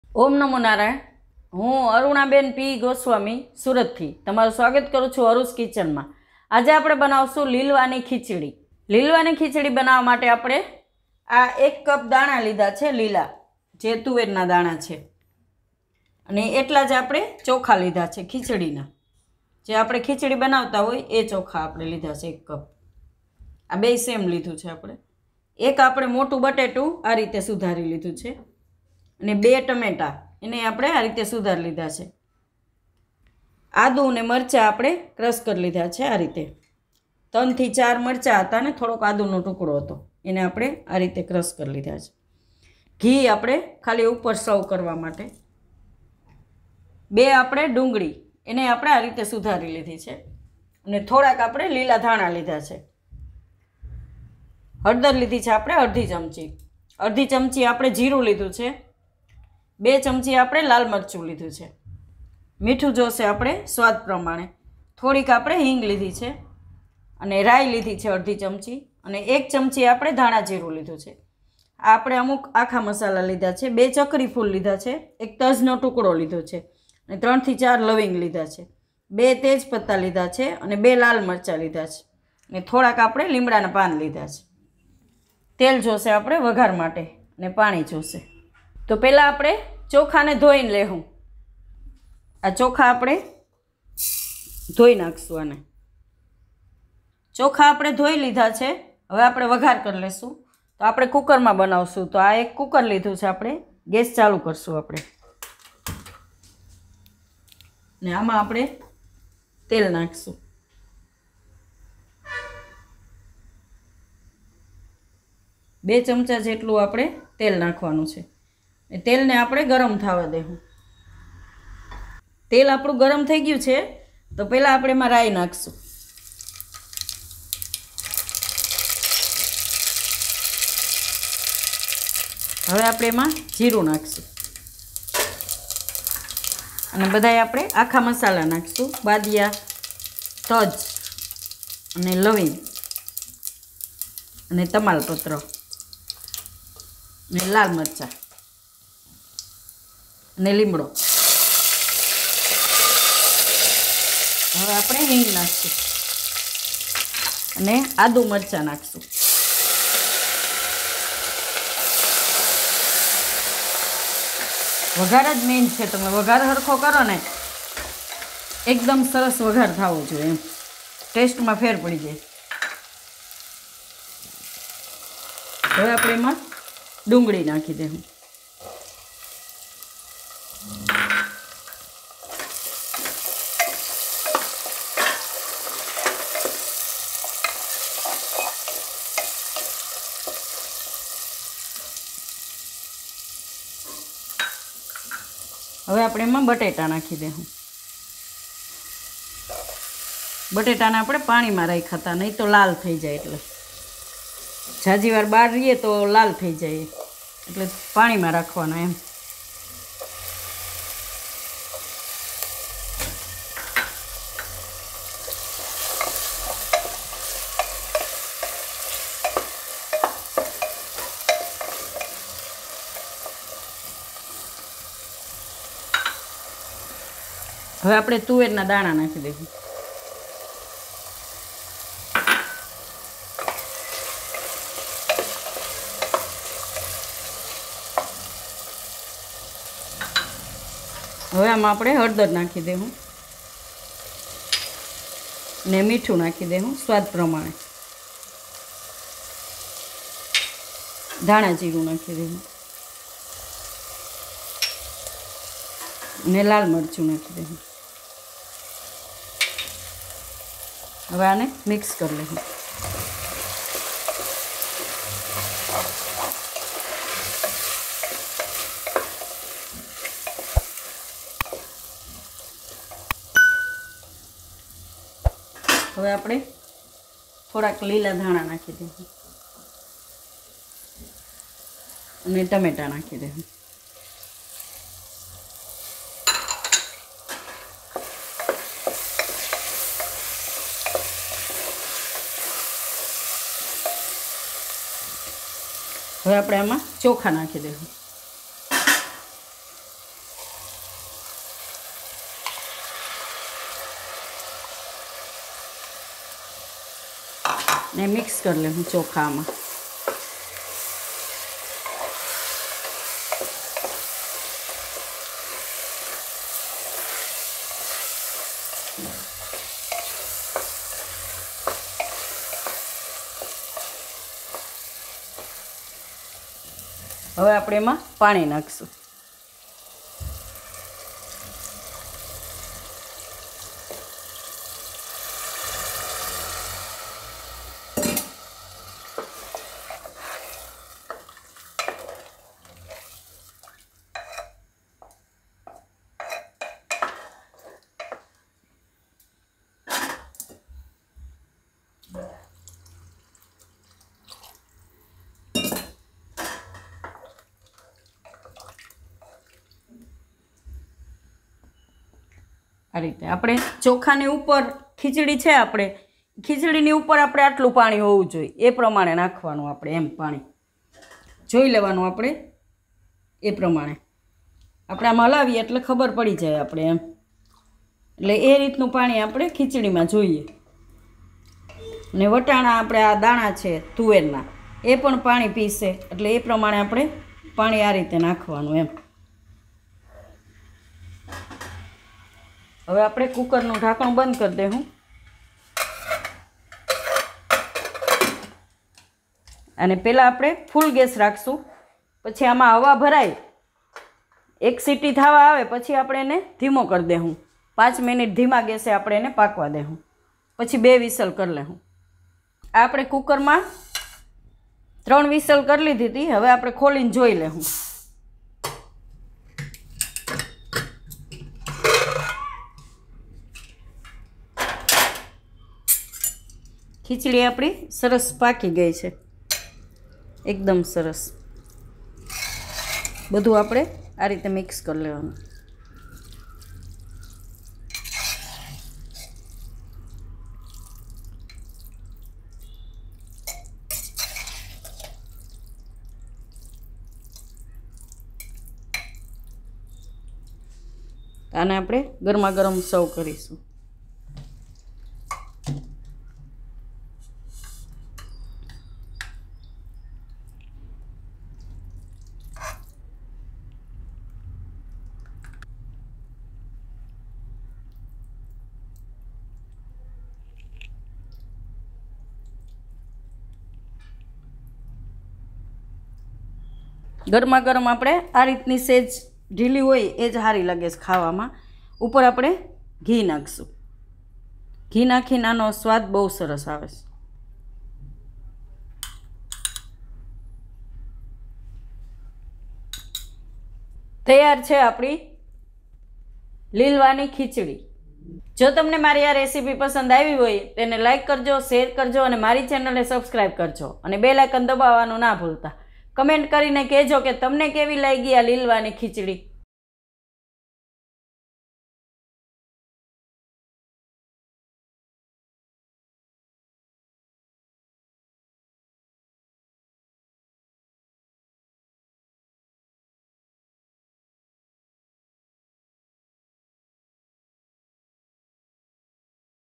લીલવા ની ખીચડી - Lilva Ni Khichdi Banavani Rit - Aru'z Kitchen - Gujarati Recipe - Winter Recipe
Welcome to Aru'z Kitchen in this video, we shall see how to make Lilva Ni Khichdi at home. Aru'z Kitchen માં આપનું સ્વાગત છે, આ વિડિઓમાં આપણે જોઈશું કે લીલવા ની ખીચડી કેવી રીતે બનાવવી.

6 in 1 Slicer and Grater